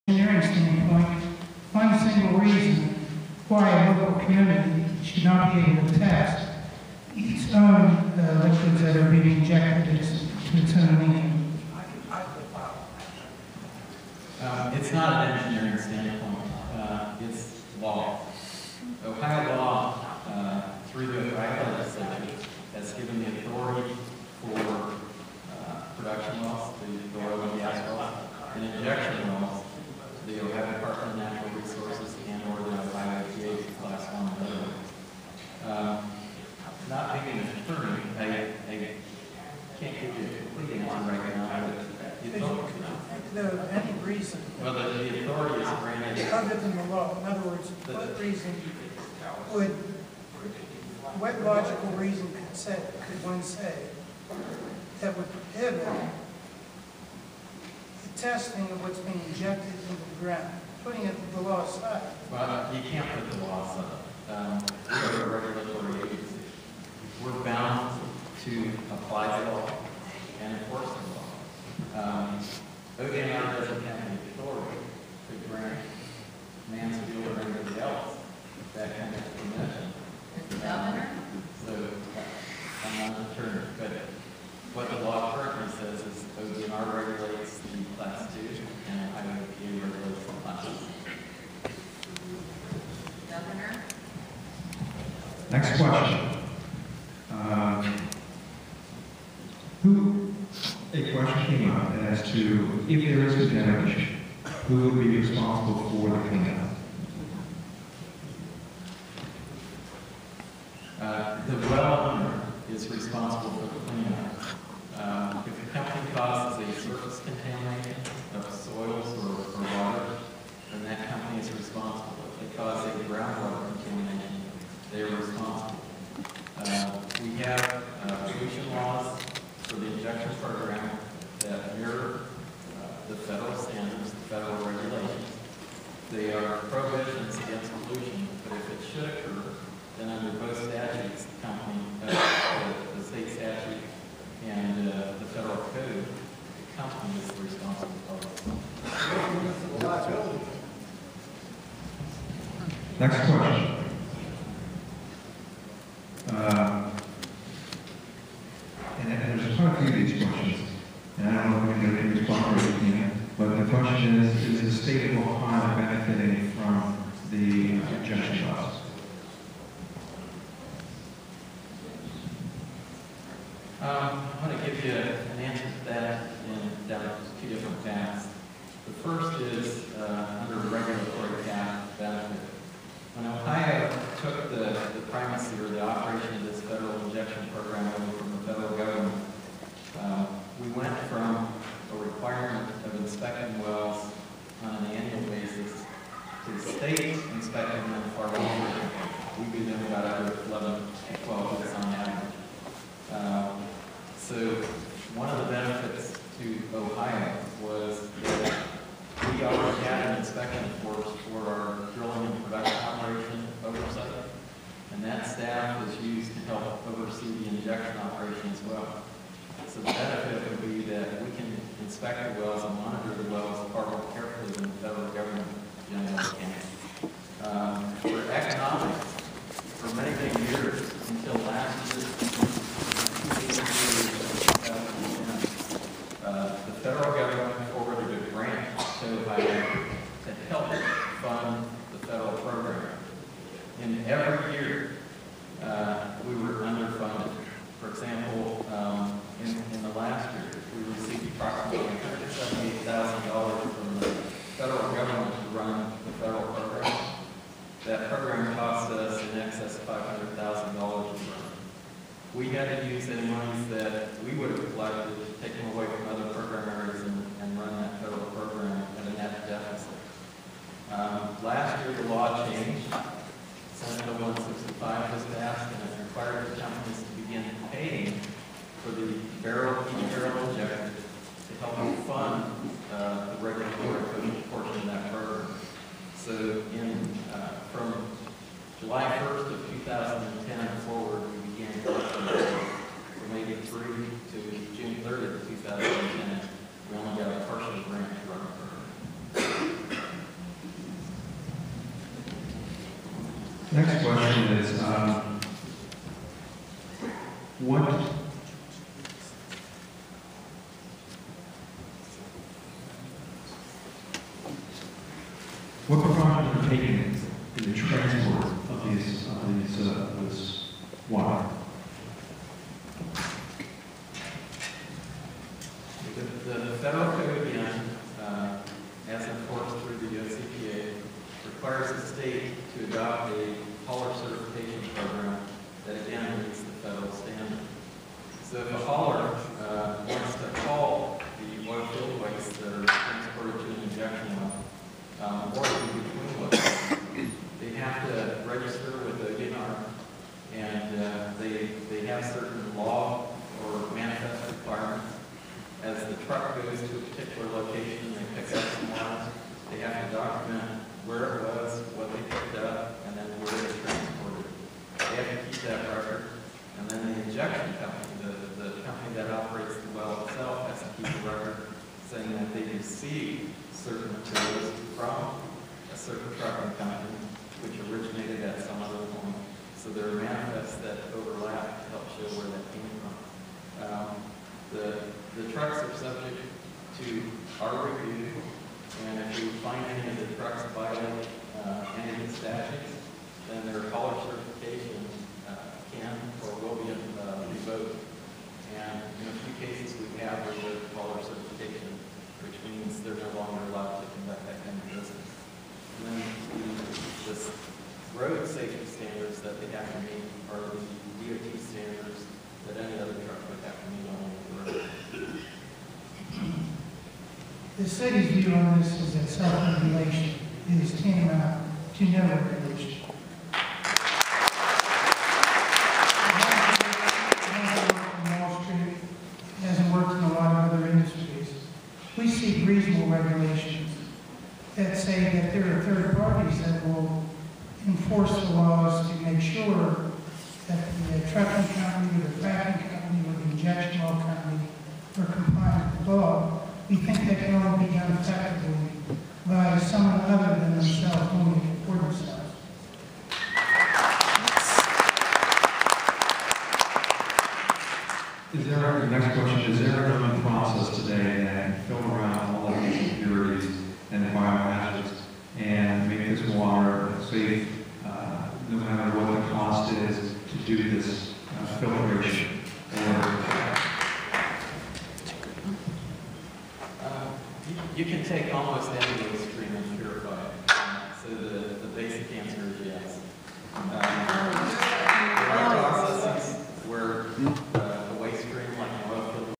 "Fracking" forum - Mansfield, Ohio (6)
Segment of public forum on "fracking" and injection wells, held at Malabar Intermediate School in Mansfield, Ohio on Jan. 19, 2012.